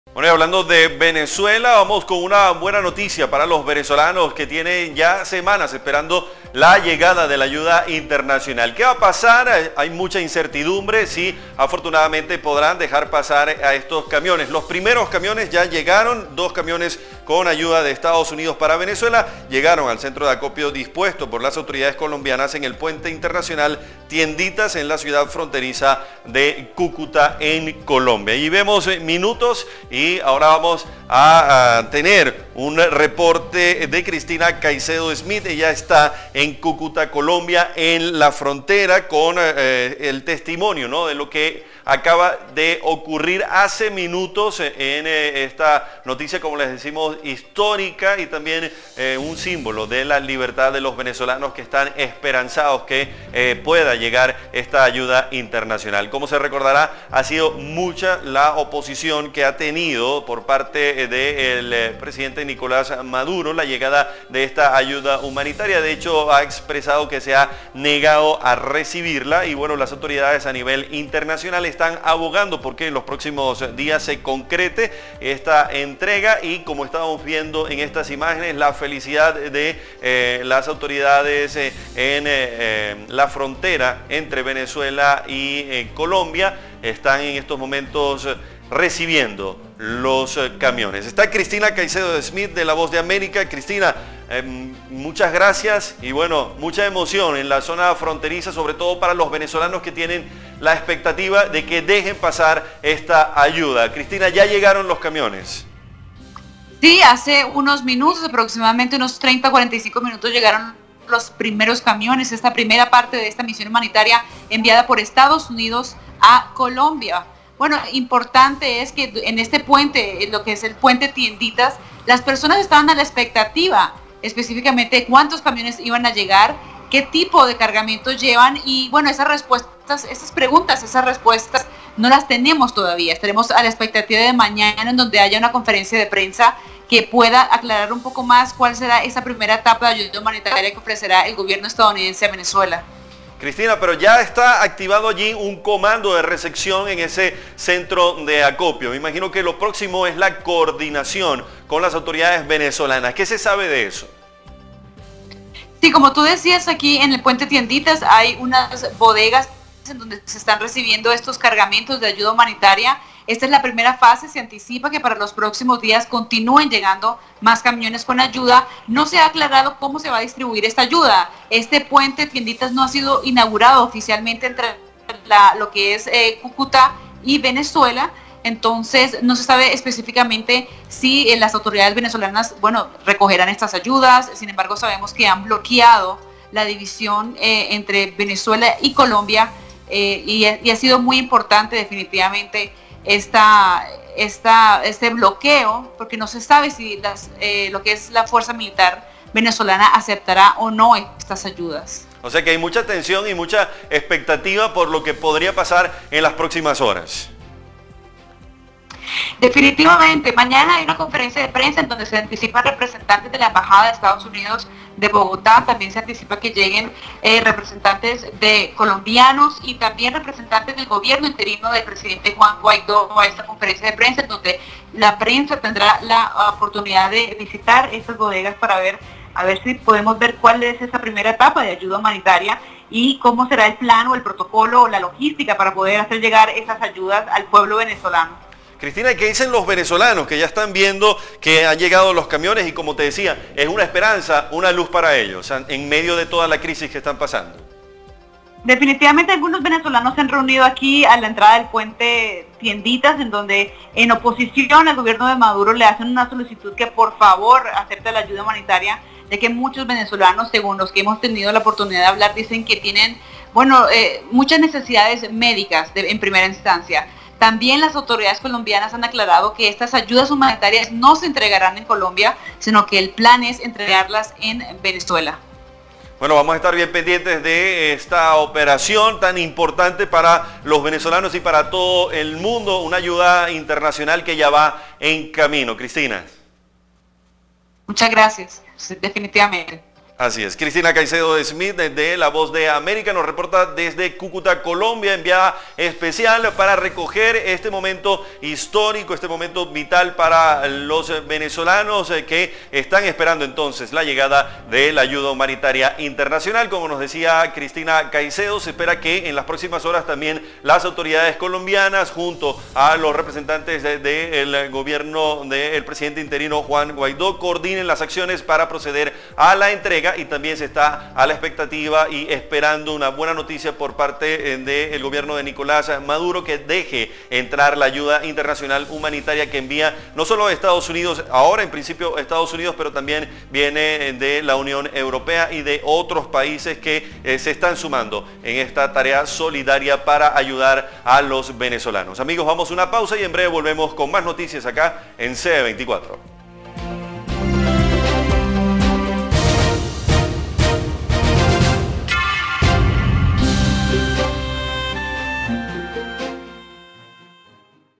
Llegan a Colombia los primeros camiones con ayuda humanitaria para venezolanos
AlertaVenezuela 🇻🇪🇨🇴| Ya están en Cúcuta-Colombia los primeros dos camiones cargados con ayuda humanitaria para la población venezolana.

Las tractomulas estarán custodiadas por funcionarios militares, a la espera de la orden para coordinar la entrega que deberá hacerse en territorio venezolano por disposición internacional.

El reporte desde la frontera colombo-venezolana, lo tiene Cristina Caicedo Smit, enviada especial de la Voz de América, para CB24, el canal de Centroamérica.

Redacción CB24

24 Horas conectados con la información